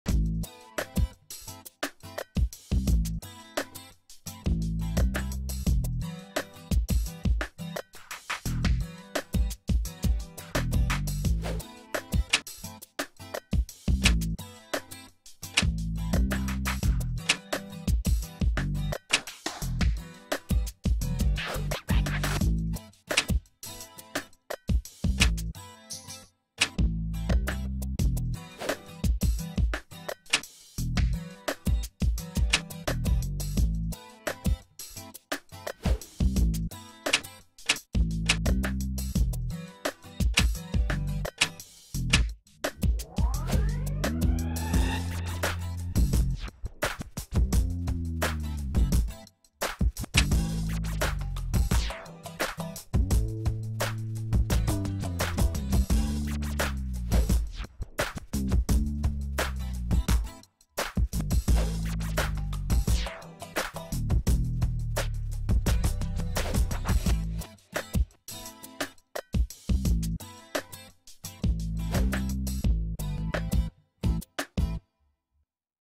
EMTEC : La clé USB iCobra
Vidéo démonstration produit de la clé  iCobra pour USB/iPhone